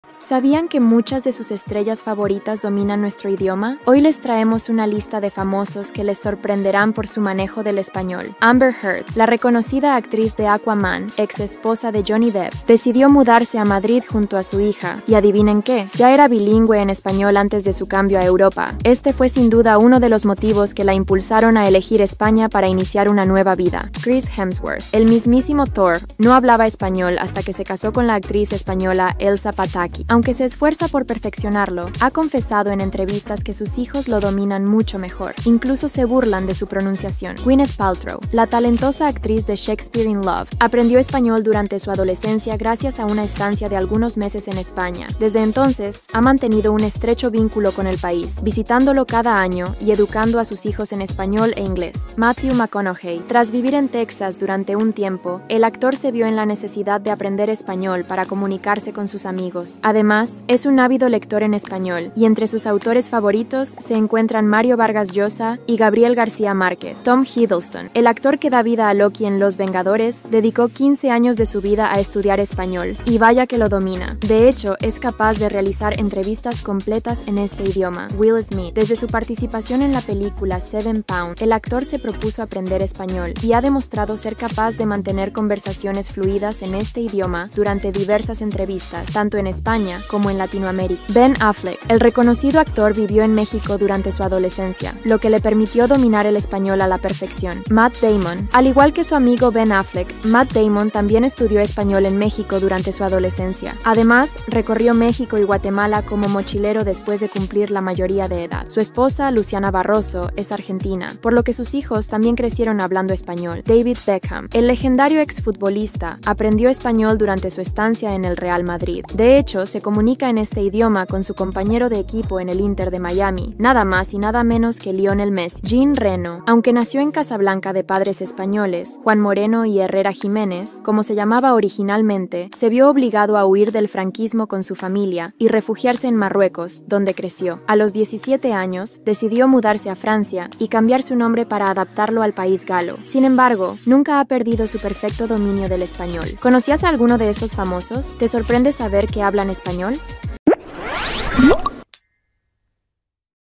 FAMOSOS que HABLAN ESPAÑOL: ¡Te sorprenderás! | HypeLine Español
¡Famosos que hablan español! Te sorprenderás (300 palabras)
¿Sabías que muchas de tus estrellas favoritas dominan nuestro idioma? Hoy te traemos una lista de algunos famosos que te sorprenderán por su manejo del español.

1. Amber Heard: La actriz de "Aquaman", exesposa de Johnny Depp, ya era bilingüe en español antes de mudarse a Madrid con su hija. ¡Este fue uno de los motivos que la impulsaron a elegir España para iniciar una nueva vida!

2. Chris Hemsworth: El mismísimo Thor, o mejor dicho, Chris Hemsworth, no hablaba español hasta que se casó con la actriz española Elsa Pataky. Aunque se esfuerza por perfeccionarlo, ha confesado en entrevistas que sus hijos lo dominan mucho mejor, ¡incluso se burlan de su pronunciación!

3. Gwyneth Paltrow: La talentosa actriz de "Shakespeare in Love" aprendió español durante su adolescencia gracias a una estancia de algunos meses en España. Desde entonces, ha mantenido un estrecho vínculo con el país, visitándolo cada año y educando a sus hijos en español e inglés.

4. Matthew McConaughey: Tras vivir en Texas durante un tiempo, el actor se vio en la necesidad de aprender español para comunicarse con sus amigos. Además, es un ávido lector en español, y entre sus autores favoritos se encuentran Mario Vargas Llosa y Gabriel García Márquez.

5. Tom Hiddleston: El actor que da vida a Loki en "Los Vengadores" dedicó 15 años de su vida a estudiar español, ¡y vaya que lo domina! De hecho, es capaz de realizar entrevistas completas en este idioma.

6. Will Smith: Desde su participación en la película "Seven Pounds", el actor se propuso aprender español, y ha demostrado ser capaz de mantener conversaciones fluidas en este idioma durante diversas entrevistas, tanto en España como en Latinoamérica.

7. Ben Affleck: El reconocido actor vivió en México durante su adolescencia, lo que le permitió dominar el español a la perfección.

8. Matt Damon: Al igual que su amigo Ben Affleck, Matt Damon también estudió español en México durante su adolescencia. Además, recorrió México y Guatemala como mochilero después de cumplir la mayoría de edad. Su esposa, Luciana Barroso, es argentina, por lo que sus hijos también crecieron hablando español.

9. David Beckham: El legendario exfutbolista aprendió español durante su estancia en el Real Madrid. De hecho, se comunica en este idioma con su compañero de equipo en el Inter de Miami, nada más y nada menos que ¡Lionel Messi!

10. Jean Reno: Aunque nació en Casablanca de padres españoles, Juan Moreno y Herrera-Jiménez, como se llamaba originalmente, se vio obligado a huir del franquismo con su familia y refugiarse en Marruecos, donde creció. A los 17 años, decidió mudarse a Francia y cambiar su nombre para adaptarlo al país galo. Sin embargo, nunca ha perdido su perfecto dominio del español.

¿Conocías a alguno de estos famosos? ¿Te sorprende saber que hablan español?

Aquí te dejo los canales de HypeLine en inglés y en portugués para que les eches un vistazo si te interesa: